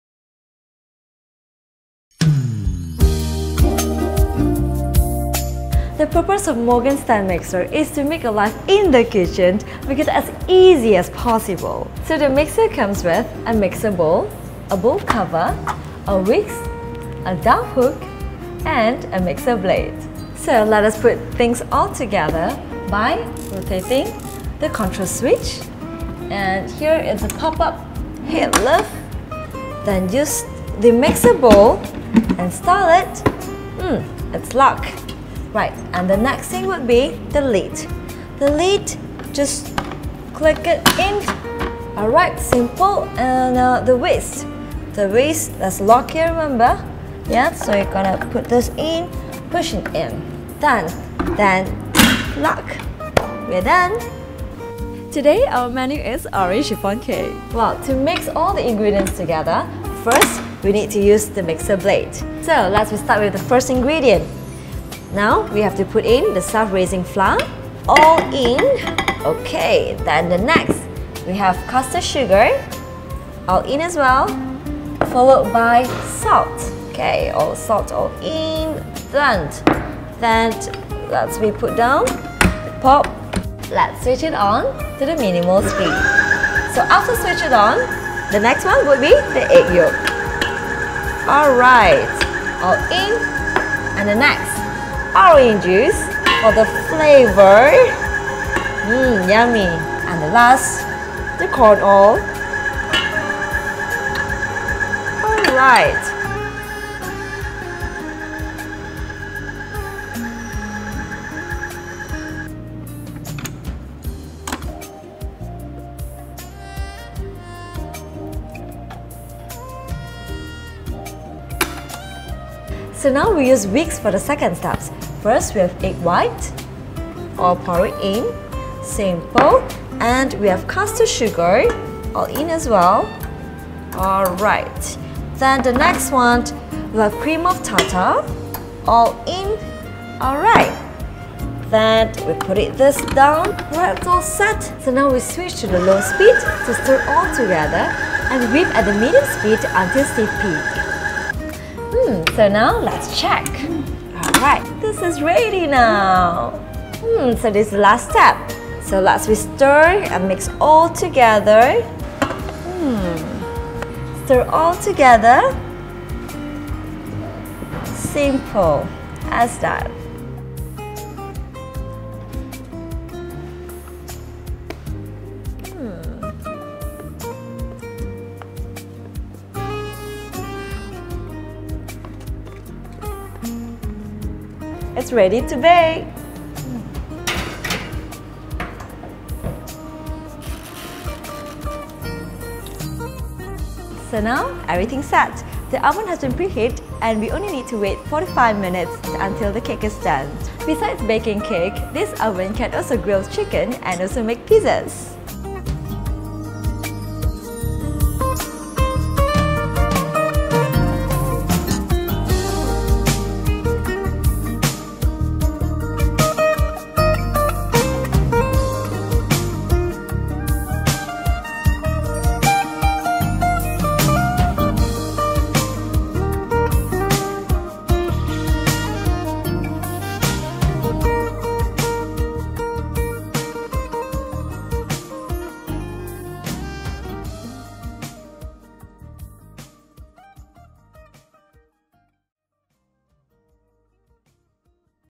MORGAN STAND MIXER
(MSM-SB1500)

-POWER RATING : 1500W POWERFUL MOTOR, LOW NOISE
- 6.7L BRUSHED STAINLESS STEEL BOWL
- VARIABLE 8 STEPLESS SPEED AND PULSE FUNCTION